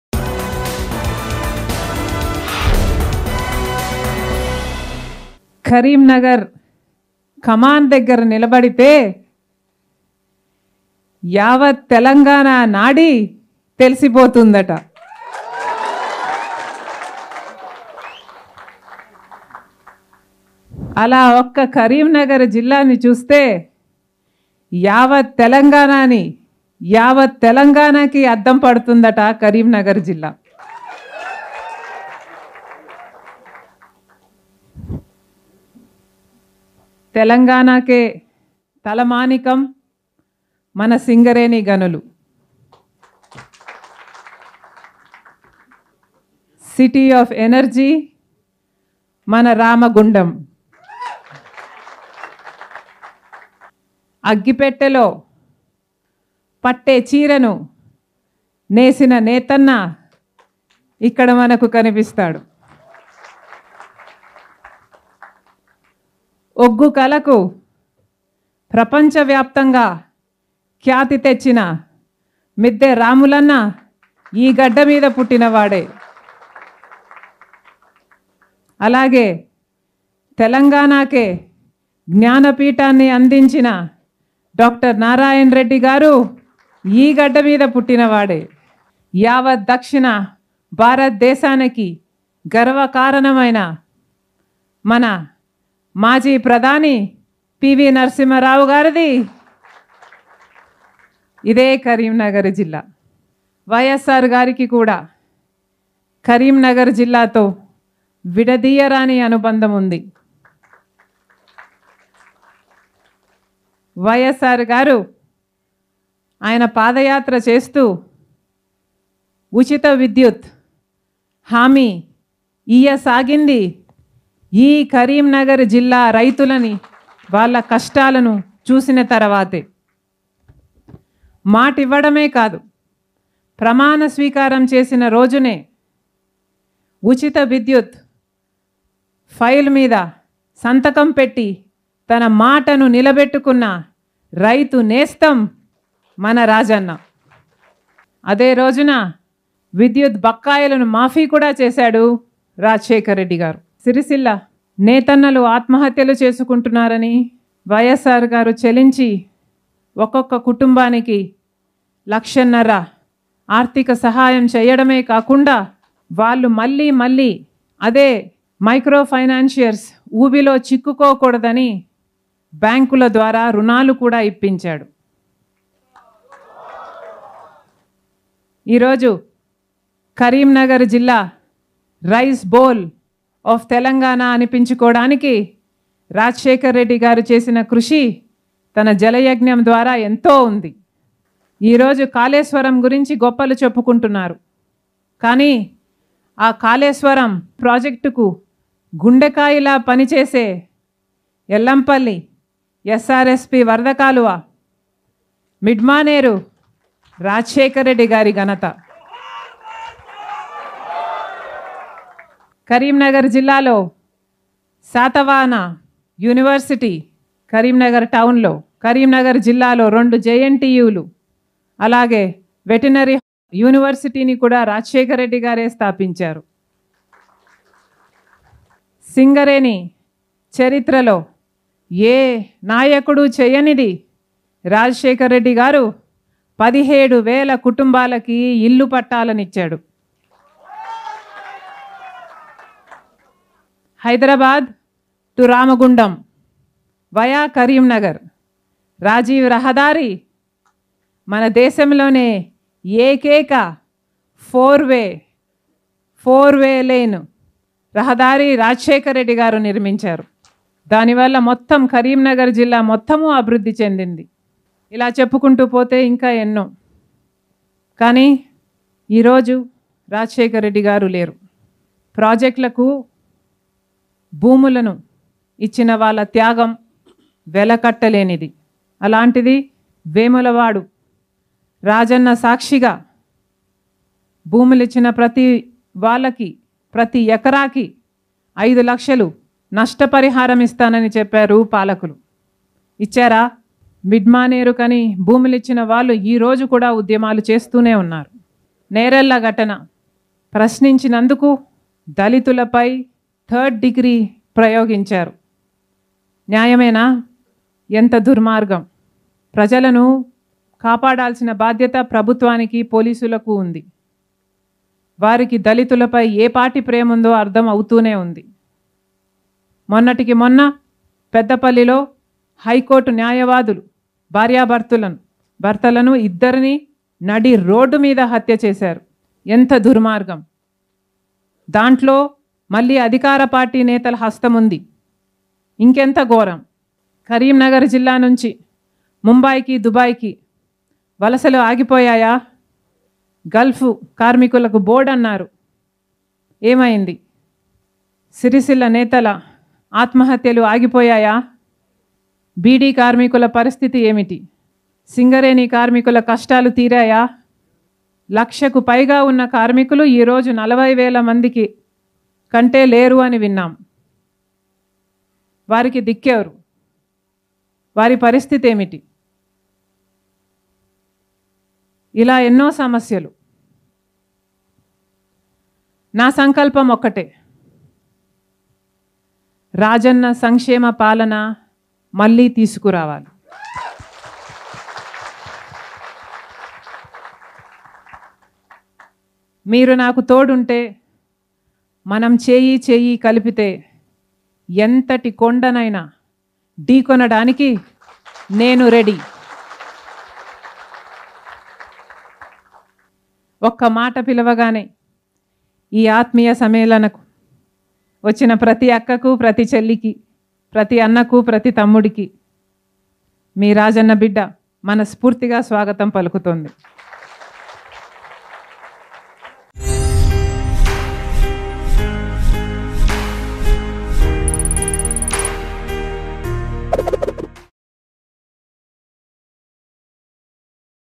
YS Sharmila Expresses Confidence Over New Party in Telangana | YSRTP | Telangana News | Mango News
YS Sharmila Expresses Confidence Over New Party in Telangana.